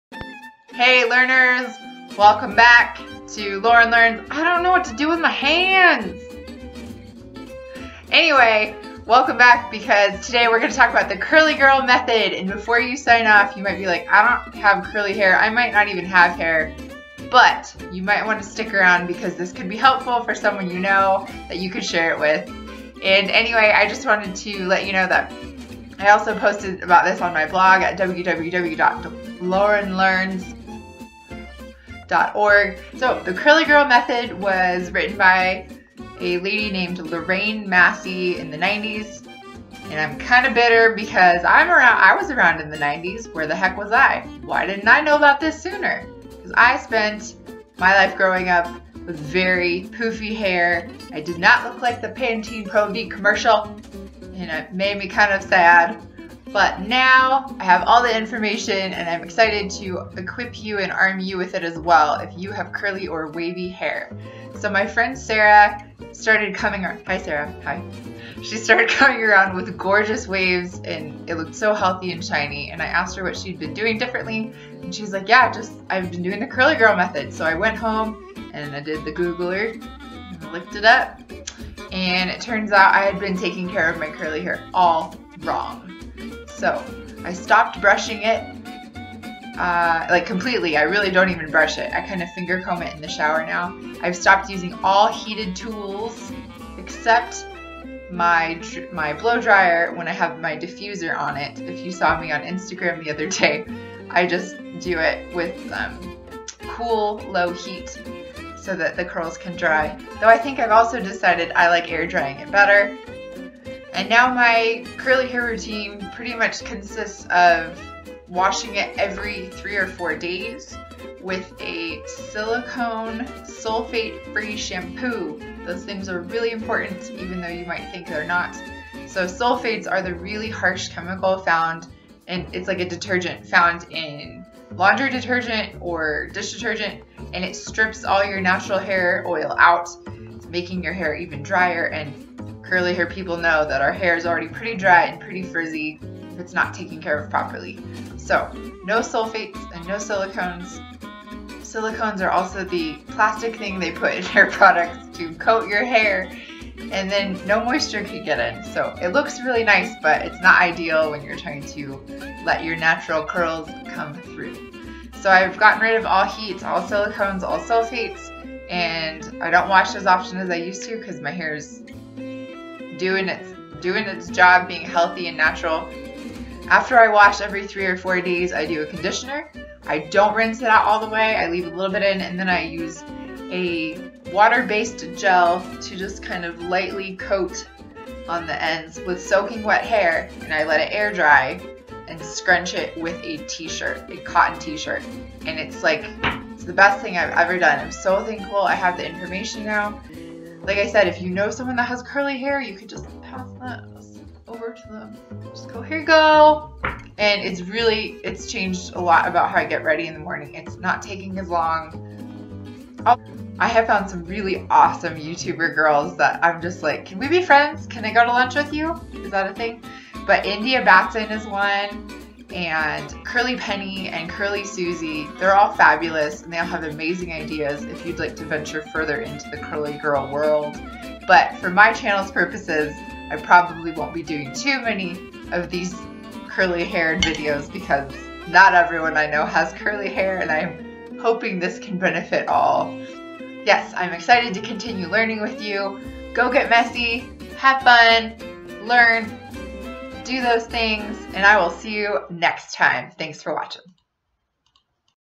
THE CURLY GIRL METHOD
Hey Learners!

Here's where I discuss a bit about the curly girl method.

CURLY HAIR YOUTUBERS:
India Batson
Curly penny
Curly Susie